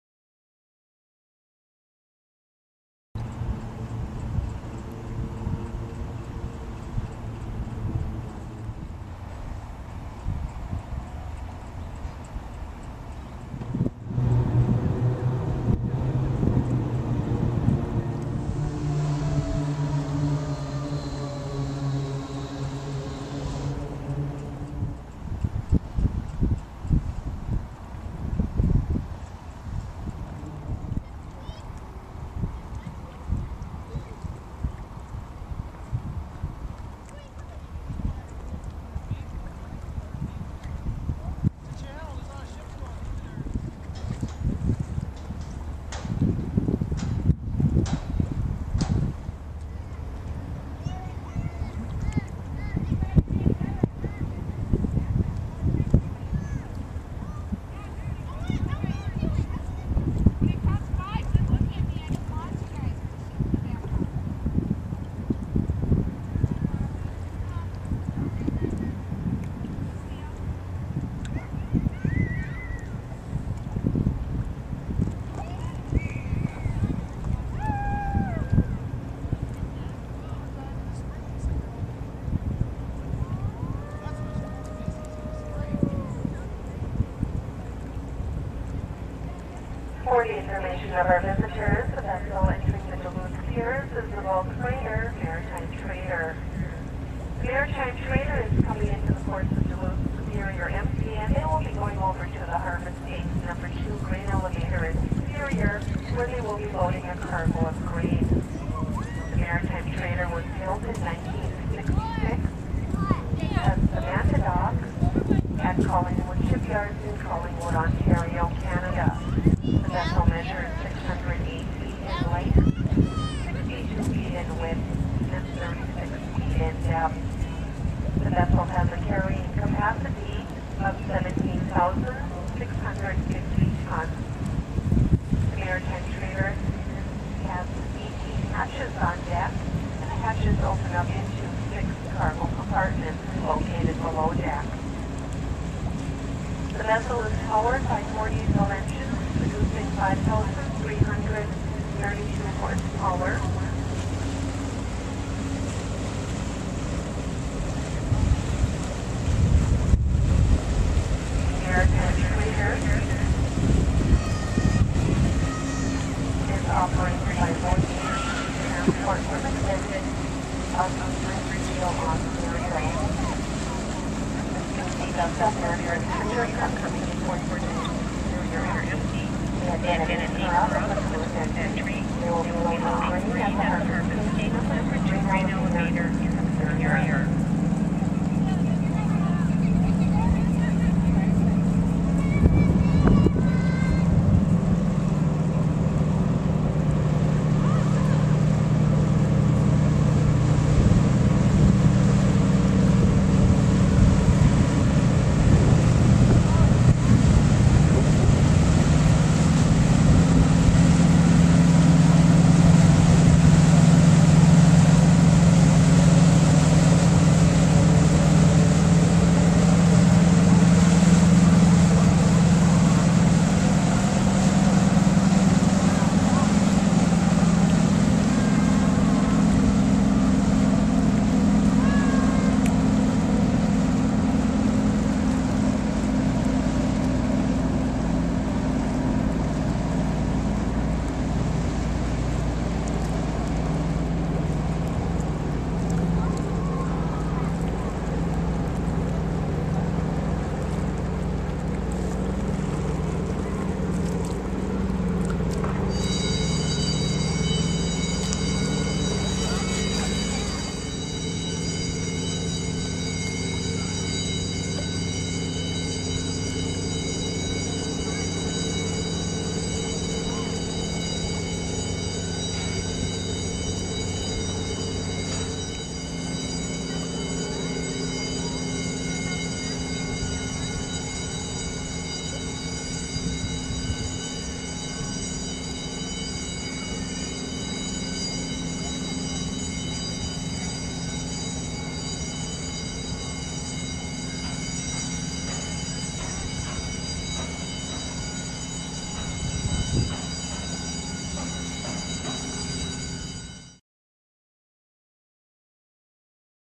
Maritime Trader enters Duluth Harbor
Created on November 29, 2010 using FlipShare.  Canadian Laker Maritime Trader arrives at Duluth to load grain.  Tug boat chasing in the harbor is either Minnesota or Kentucky.